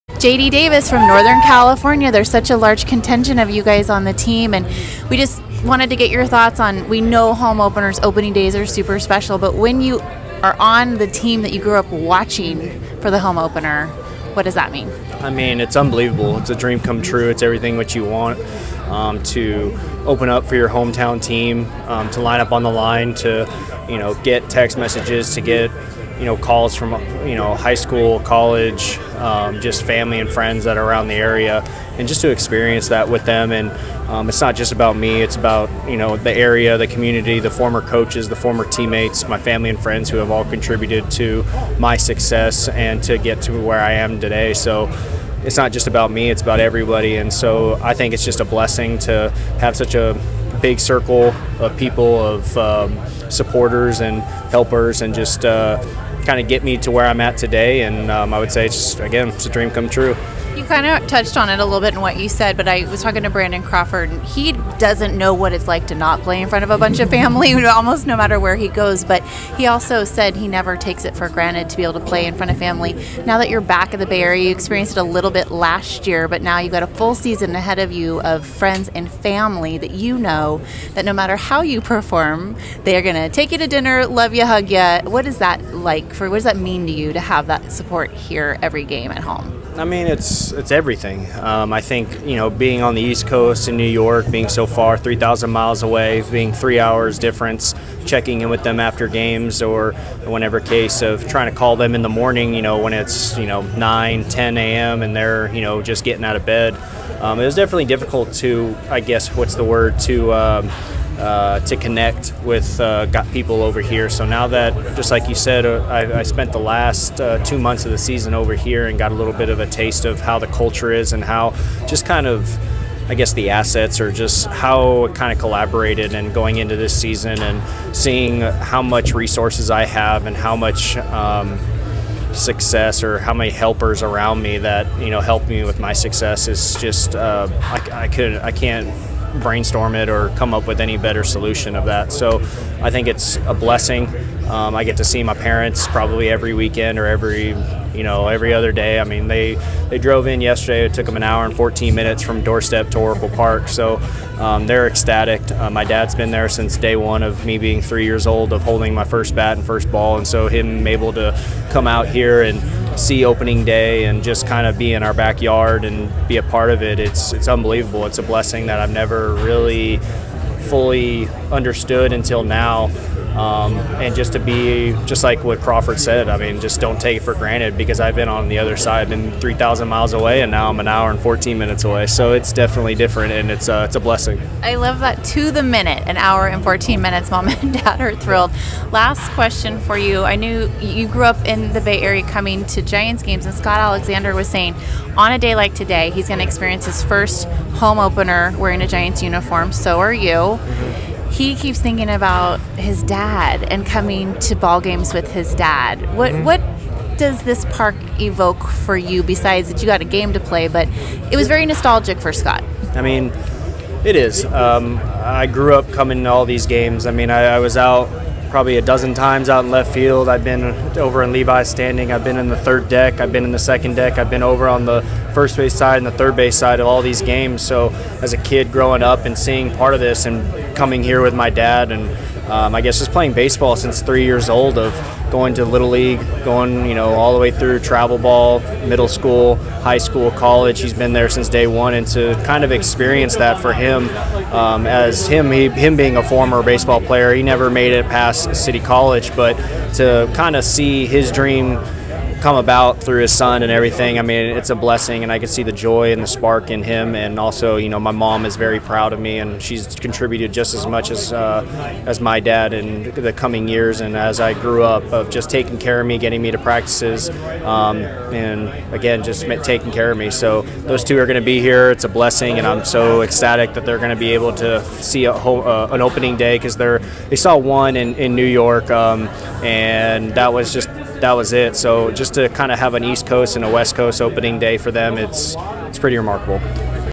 SF Giants JD Davis
JD Davis talks about experiencing his first Home Opener as a Giant.